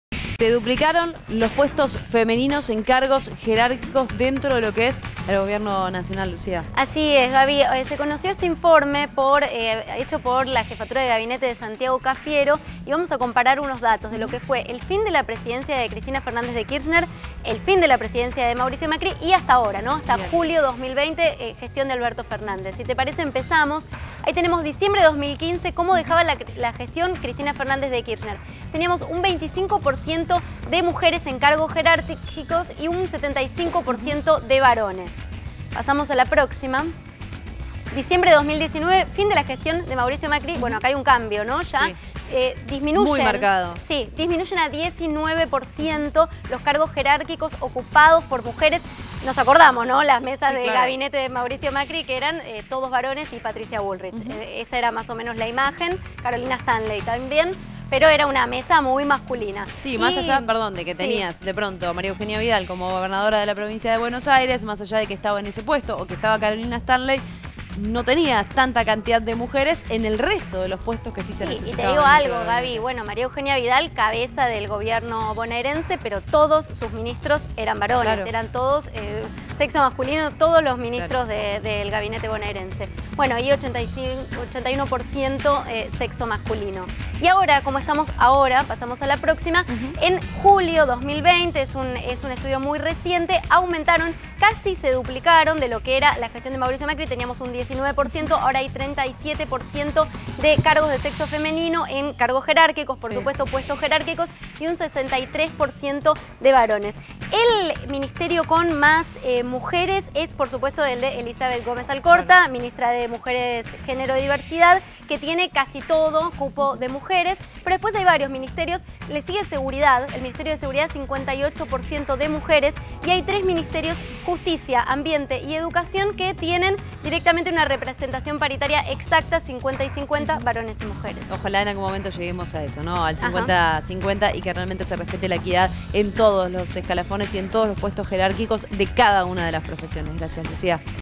Más mujeres en cargos jerárquicos del Gobierno
Las mujeres avanzan posiciones también en la política: ocupan más de uno de cada tres puestos jerárquicos del gabinete nacional, una cifra récord para la historia argentina.